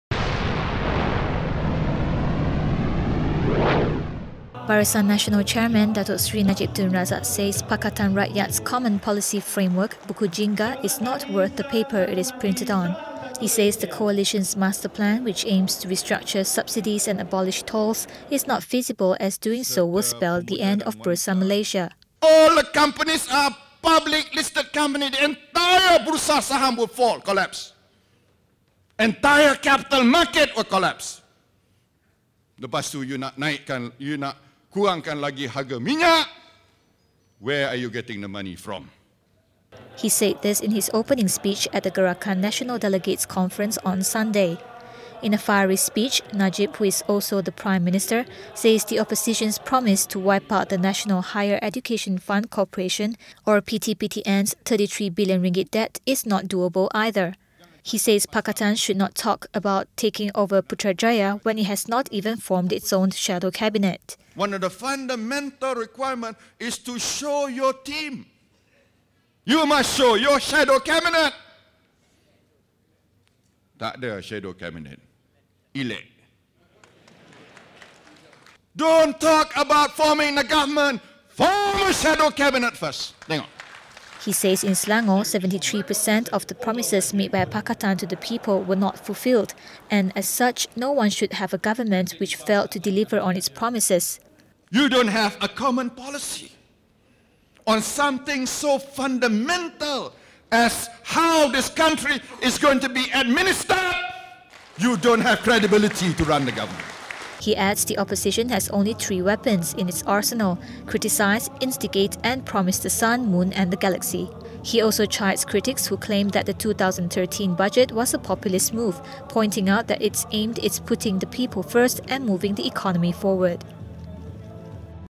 PM to Pakatan: Form shadow cabinet first
Prime Minister Datuk Seri Najib Tun Razak says Pakatan Rakyat should not talk about taking over the federal government when it has not even formed its own shadow cabinet.